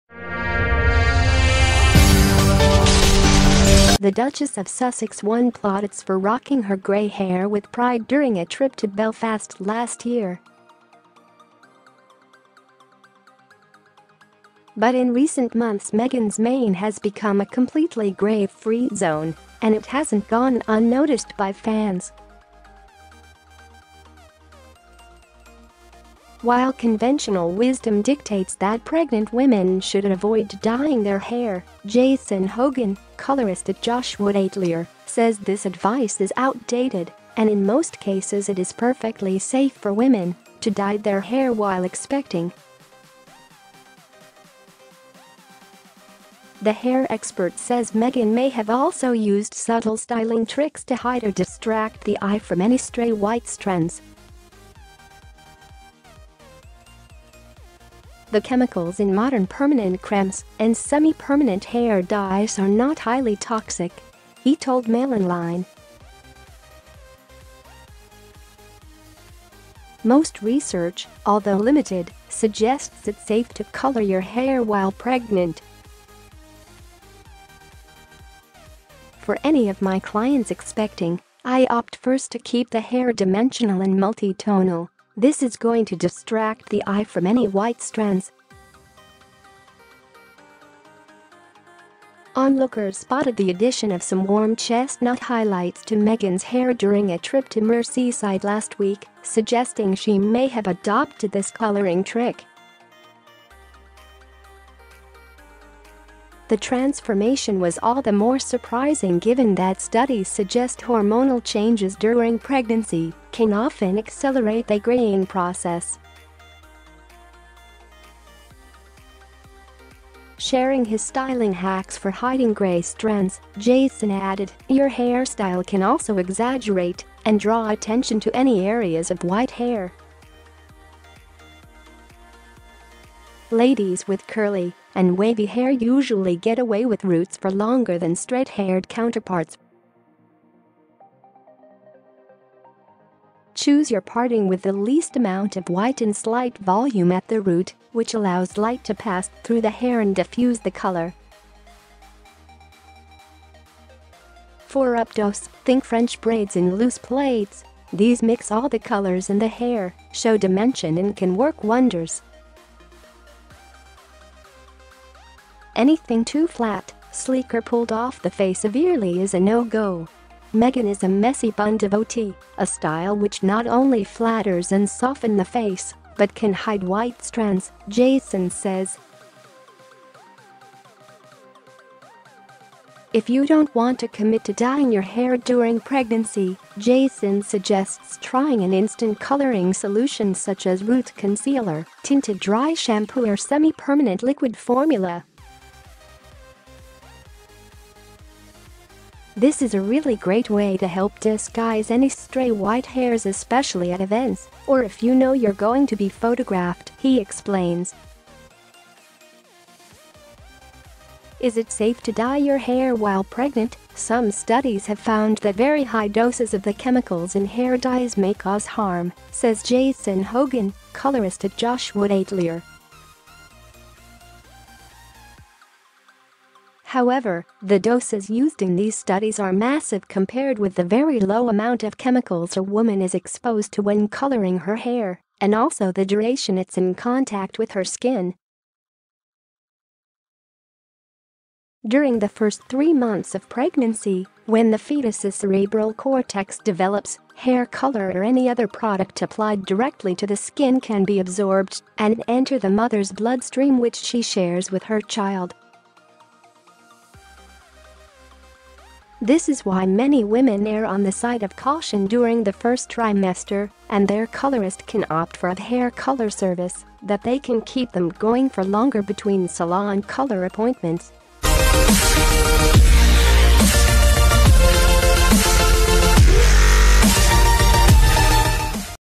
Meghan Fashion - How Meghan Markle has avoided grey hair during pregnancy
The Duchess of Sussex won plaudits for rocking her grey hair with pride during a trip to Belfast last year. But in recent months Meghan's mane has become a completely grey-free zone — and it hasn't gone unnoticed by fans.While conventional wisdom dictates that pregnant women should avoid dyeing their hair, Jason Hogan, colourist at Josh Wood Atelier, says this advice is outdated and in most cases it is perfectly safe for women to dye their hair while expecting.  The hair expert says Meghan may have also used subtle styling tricks  to hide or 'distract the eye' from any stray white strands.  'The chemicals in modern permanent cremes and semi-permanent hair dyes are not highly toxic,' he told MailOnline.'Most research, although limited, suggests it's safe to colour your hair while pregnant.'  'For any of my clients expecting, I opt first to keep the hair dimensional and multi tonal; this is going to distract the eye from any white strands.'Onlookers spotted the addition of some warm chestnut highlights to Meghan's hair during a trip to Merseyside last week, suggesting she may have adopted this colouring trick.The transformation was all the more surprising given that studies suggest hormonal changes during pregnancy can often accelerate they greying process. Sharing his styling hacks for hiding grey strands, Jason added: 'Your hairstyle can also exaggerate and draw attention to any areas of white hair. 'Ladies with curly and wavy hair usually get away with roots for longer than straight-haired counterparts.'Choose your [parting] with the least amount of white and slight volume at the root, which allows light to pass through the hair and diffuse the colour.'For up-dos think French braids and loose plaits; these mix all the colours in the hair, show dimension and can work wonders. Anything too flat, sleek or pulled off the face severely is a no go.'  Meghan is a messy bun devotee, a style which not only flatters and soften the face but can hide white strands, Jason says. If you don't want to commit to dyeing your hair during pregnancy, Jason suggests trying an instant colouring solution such as root concealer, tinted dry shampoo or semi-permanent liquid formula. 'This is a really great way to help disguise any stray white hairs,especially at events or if you know you're going to be photographed,' he explains.   Is it safe to dye your hair while pregnant?   'Some studies have found that very high doses of the chemicals in hair dyes may cause harm,' says Jason Hogan, colourist at Josh Wood Atelier. 'However, the doses used in these studies are massive compared with the very low amount of chemicals a woman is exposed to when colouring her hair and also the duration it’s in contact with her skin.'During the first three months of pregnancy — when the foetus's cerebral cortex develops — hair colour or any other product applied directly to the skin can be absorbed and enter the mother's blood stream which she shares with her child. 'This is why many women er